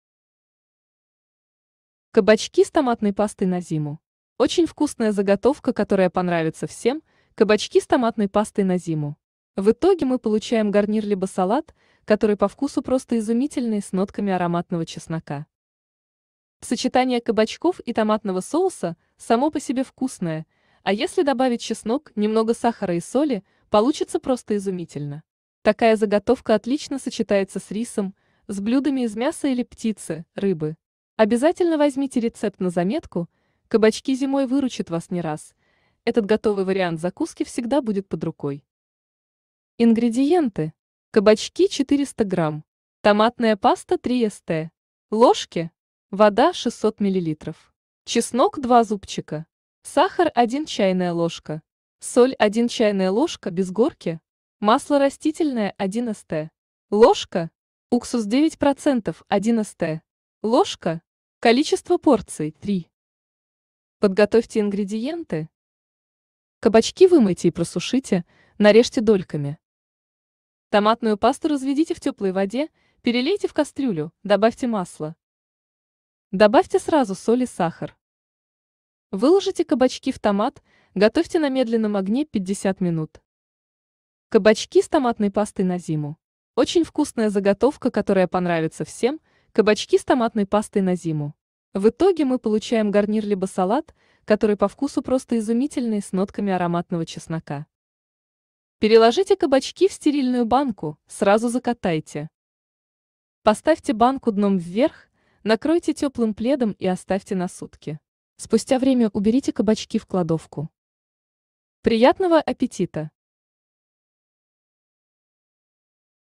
Кабачки с томатной пастой на зиму
Очень вкусная заготовка, которая понравится всем — кабачки с томатной пастой на зиму. В итоге мы получаем гарнир либо салат, который по вкусу просто изумительный, с нотками ароматного чеснока.